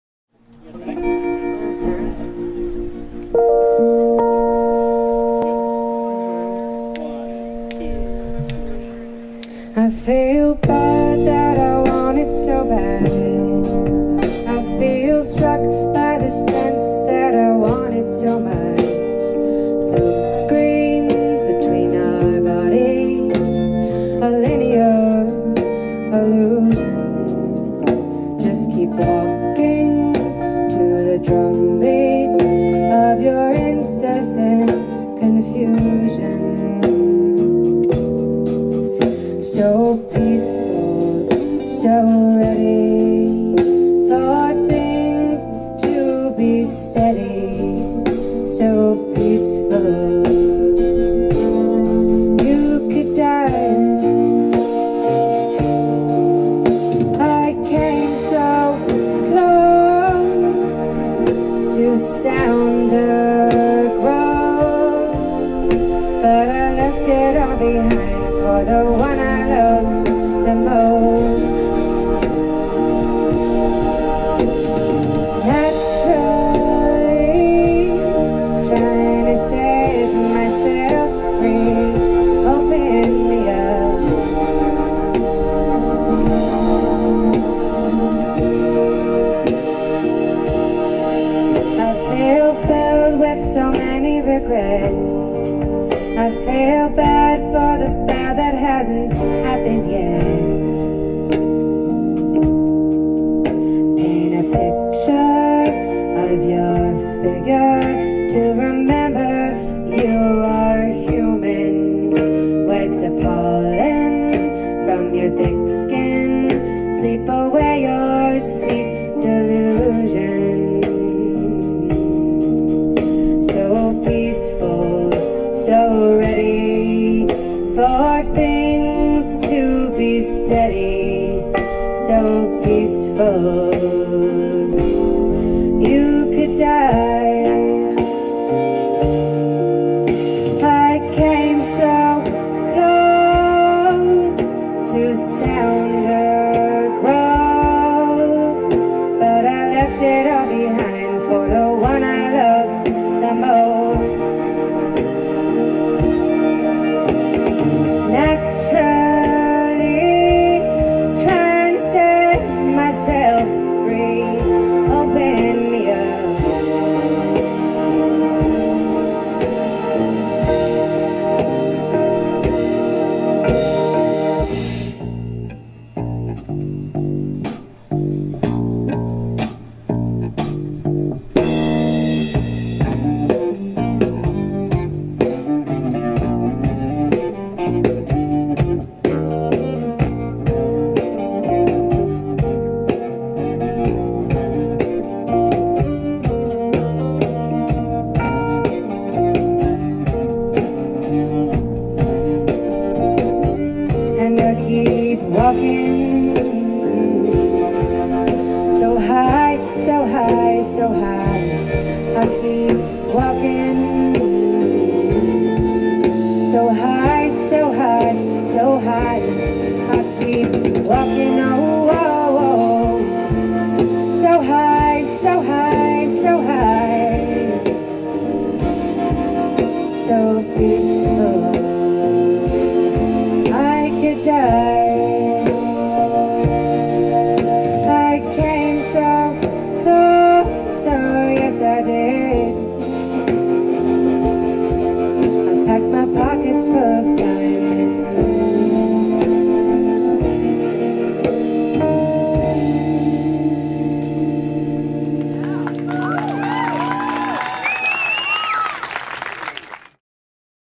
Lotion Princess at Fort Reno 2018 - Part 1 - July 2, 2018
Lotion Princess from Washington, DC starts things off at this year's Fort Reno concert series with a beautiful set that actually made the stifling heat and humidity that evening seem kinda nice.